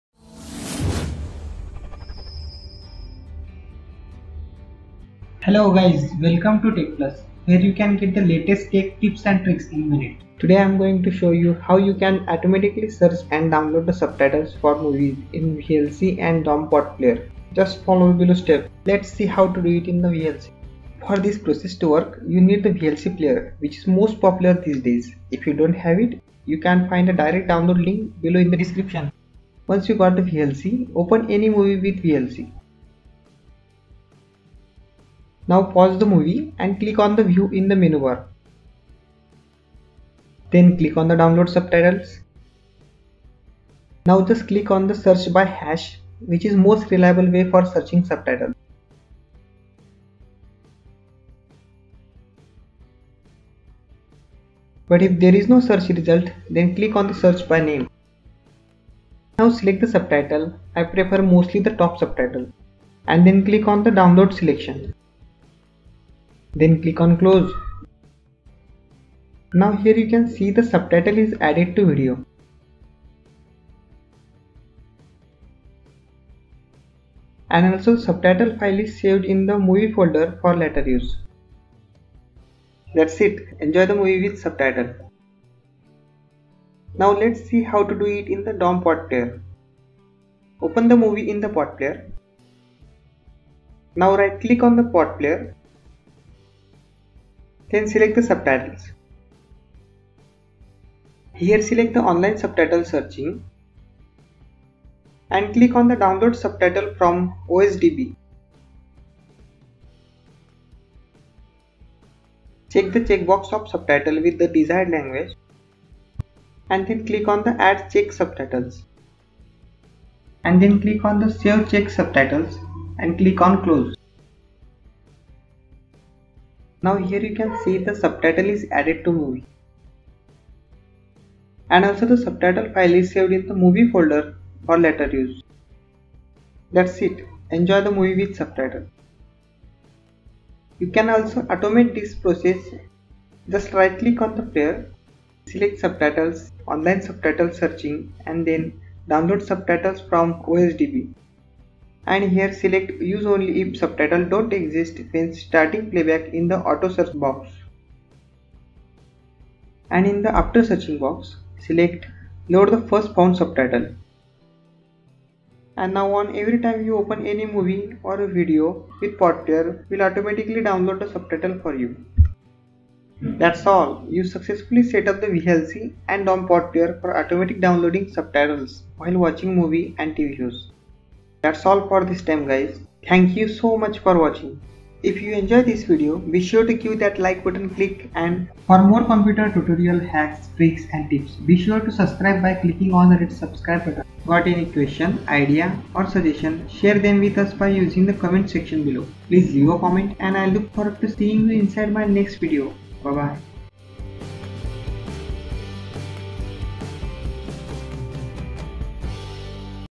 How To Automatically Search and Download Subtitles For Movies in VLC and Daum PotPlayer NEW!!! 2016✔
Today I'm going to show you, how you can automatically Search and Download Subtitles For Movies in VLC and Daum PotPlayer
Please Give it a View : )